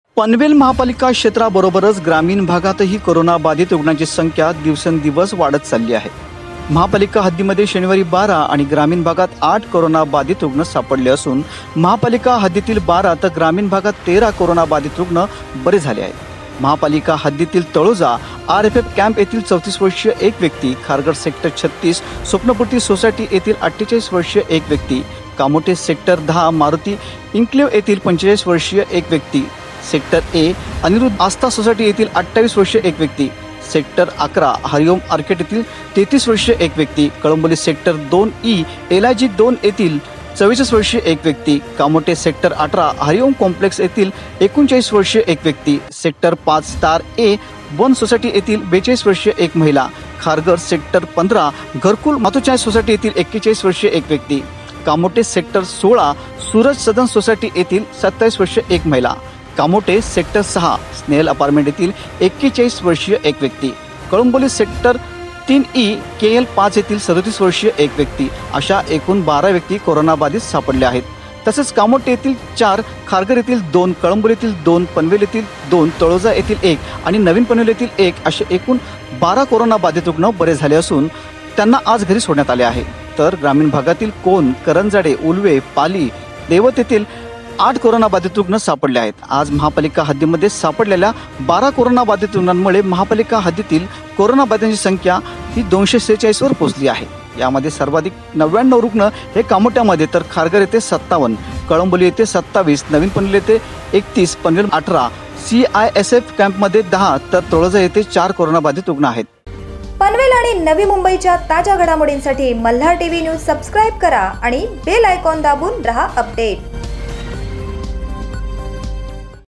malhartvnews | पनवेल तालुक्यात 20 कोरोनाचे नवीन रुग्ण !
malhartvnews at panvel | पनवेल तालुक्यात 20 कोरोनाचे नवीन रुग्ण;तळोजा रॅपिड अॅक्शन फोर्स मध्येही कोरोनाचा शिरकाव;दोन ऑर्थर रोड जेल पोलिसांनाही कोरोनाची लागण !